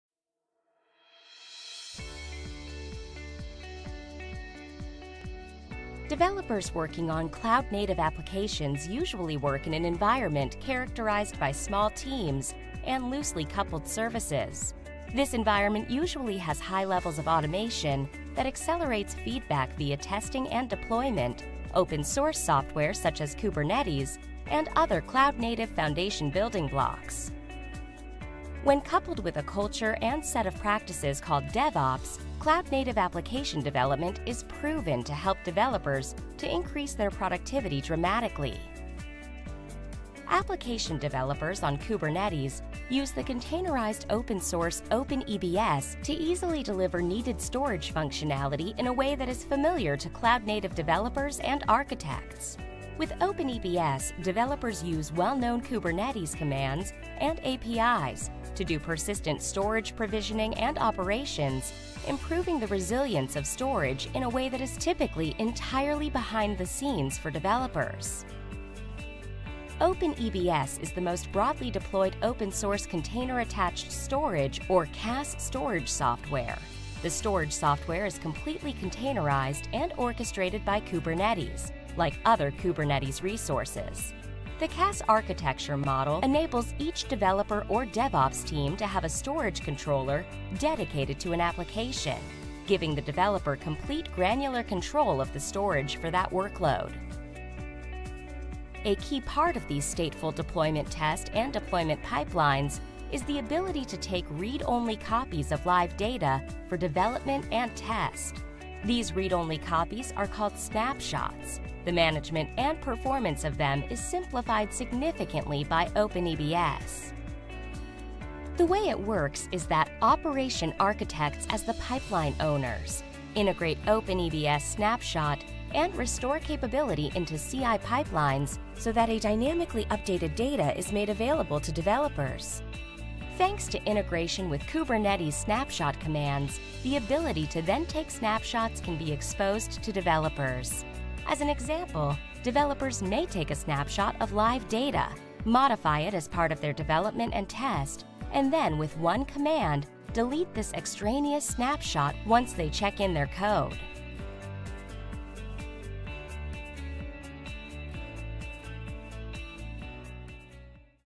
What is OpenEBS?
OpenEBS is the most active Container Attached Storage project with the biggest user base and community. It enables your DevOps teams to have their own storage policies for every workload.
OpenEBS is truly Kubernetes native and 100% in user space. It is not a Scale-out Storage System that requires a storage admin. If you know how to use K8s, you know how OpenEBS works